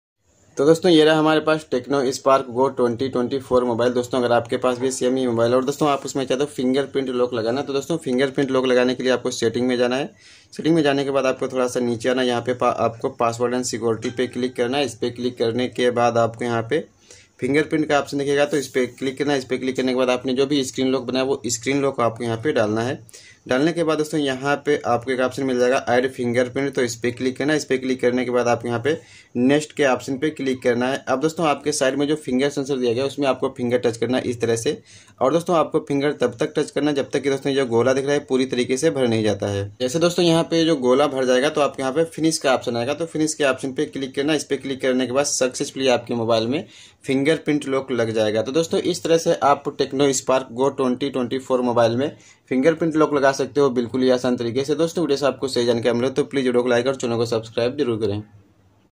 tecno spark go 2024 me fingerprint lock kaise lagaye | how to set finger lock tecno spark go 2024
hello friends aaj ki is video ke madhyam se Maine aap logo ko bataya hai ki tecno spark go 2024 mobile me fingerprint lock kaise lagaya jata hai